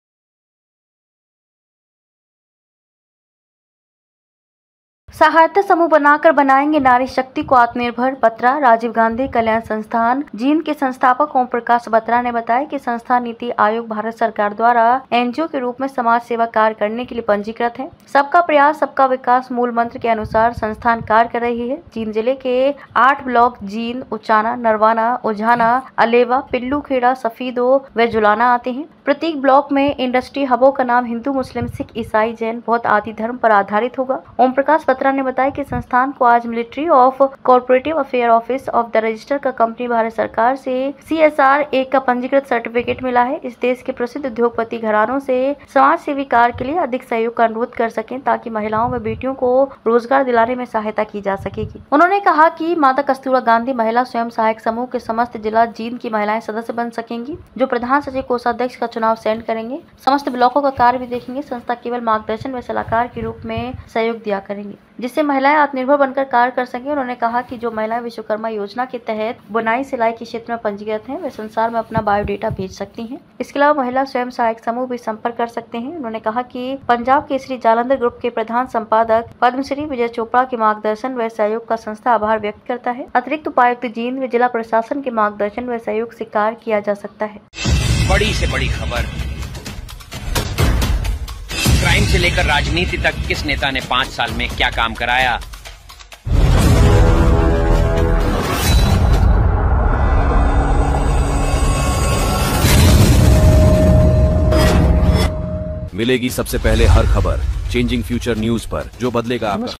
Empowering Women in Jind, Haryana: Formation of Support Groups for Self-Reliance"
In a progressive move, support groups are being established in Jind, Haryana, aimed at fostering self-reliance among women. Explore how these initiatives empower women economically and socially, marking a significant step towards gender equality and empowerment.